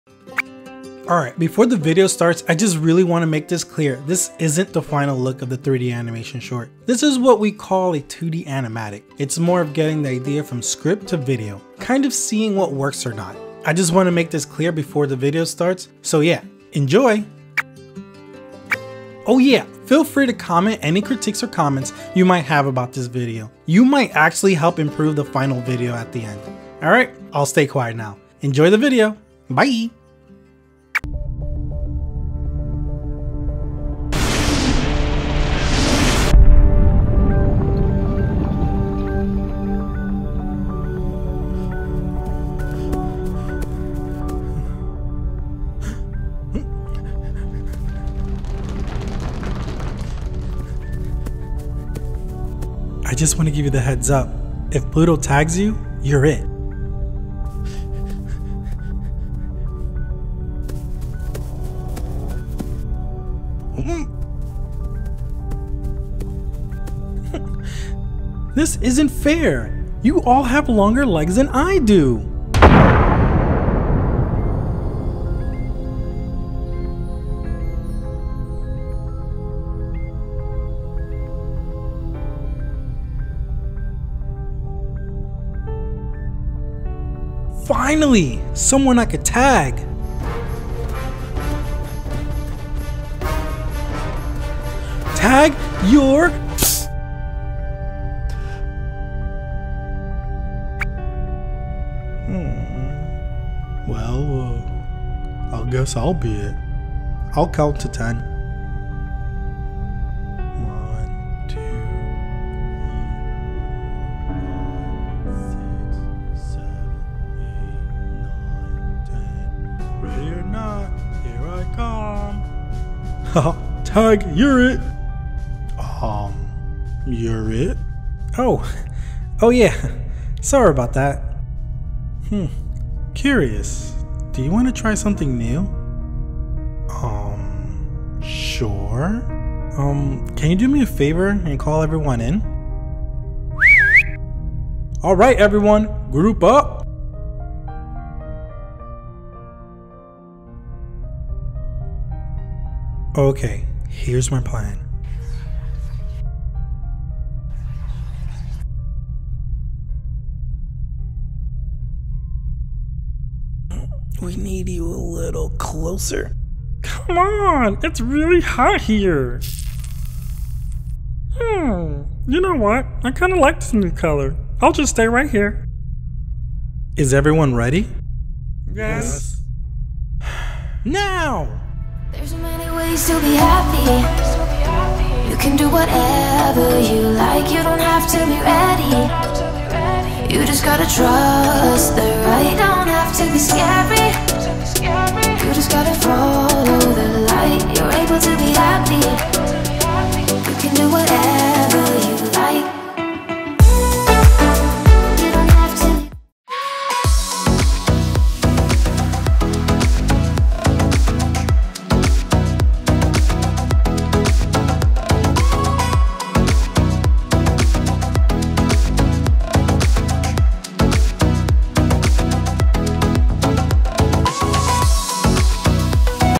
Solar System - 2DAnimatic - Animation Short Film
This is the 2D animatic for my next animation short film "Solar System". A fun little story on how the planets started rotating around the sun creating the solar system we know today.  Take a look and let me know what you think so far.